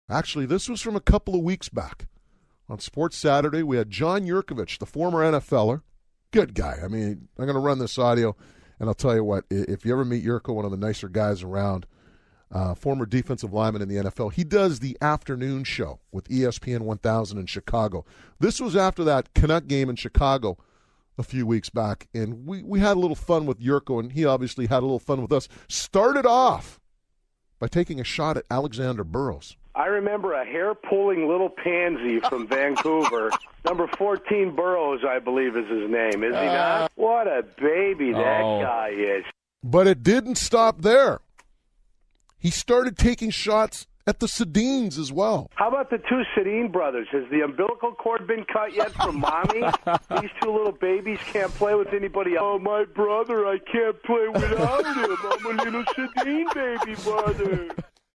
John Jurkovic makes fun of the Sedins + Burrows
Bob Marjanovich plays the clip of John Jurkovic of ESPN 1000 after the March 29 4-0 Canucks victory in Chicago.